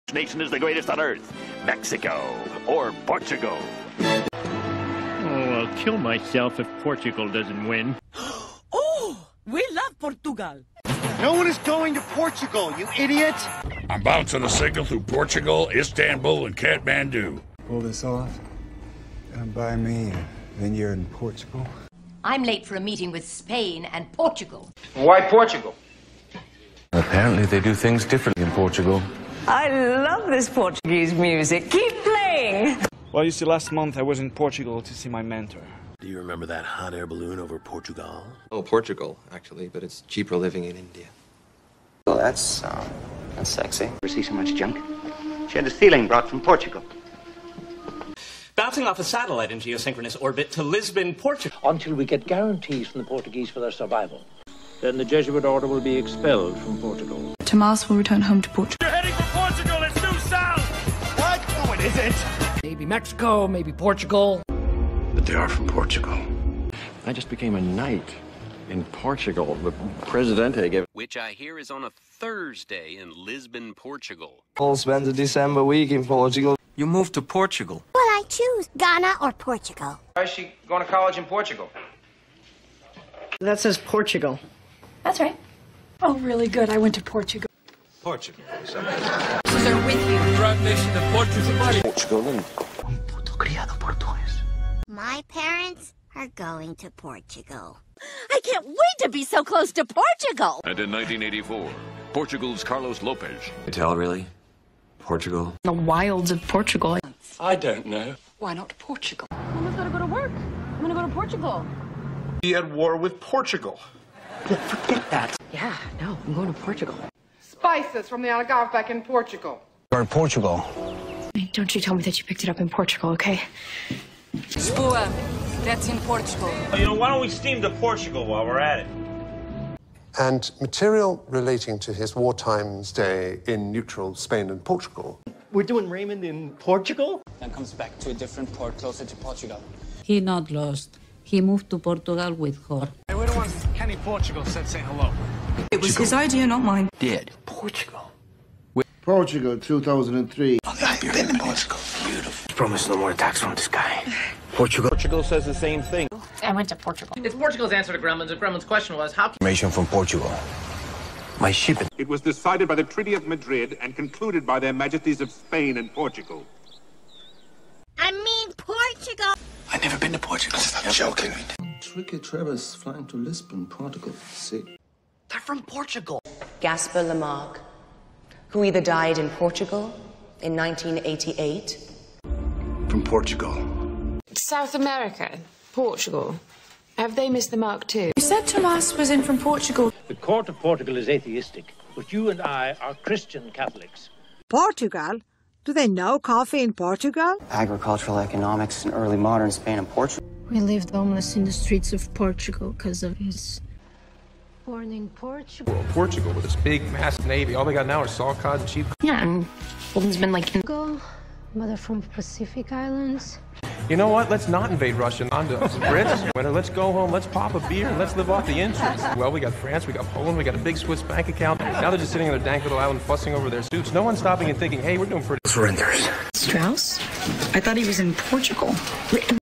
PORTUGAL mentioned in AMERICAN movies and series
PORTUGAL mentioned in AMERICAN movies and series mashup